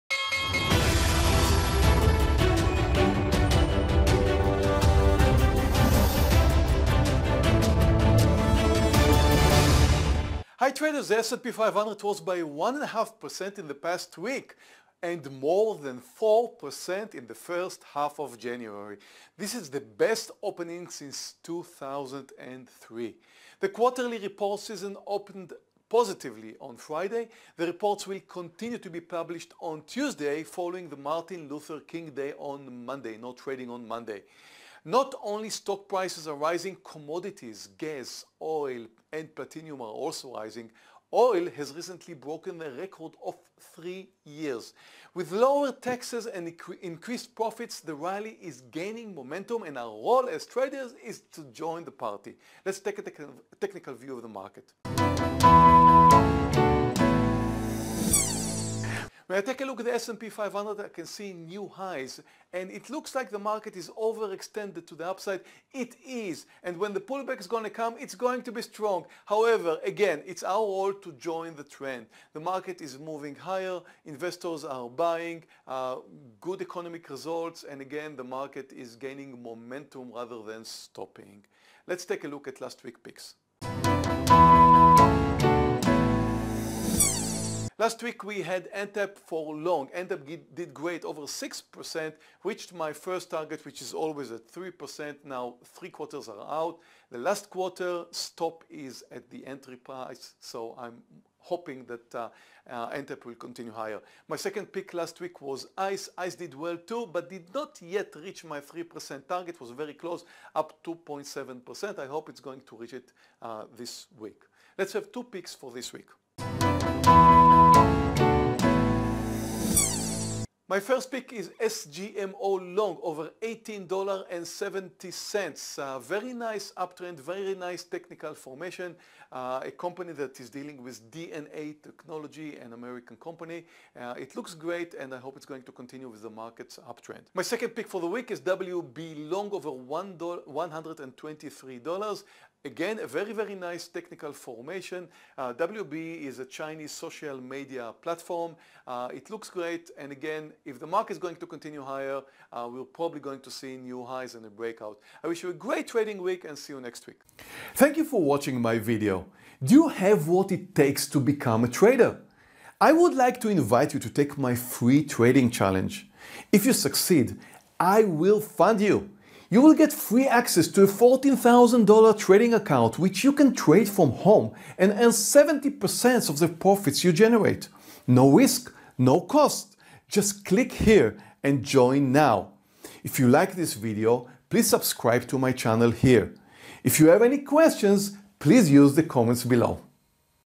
Day Trading Stock Picks & Weekly Review - January 15th.
Hi traders, the S&P 500 rose 1.5% in the past week, and more than 4% in the first half of January. The best opening since 2003! The quarterly reports season opened positively on Friday. The reports will continue to be published on Tuesday following Martin Luther King's day off on Monday. Not only stock prices are rising; commodities: gas, oil and platinum are also rising. Oil has recently broken a record of 3 years! With lower taxes and increased profits, the rally is gaining momentum and our role as traders is to join the party!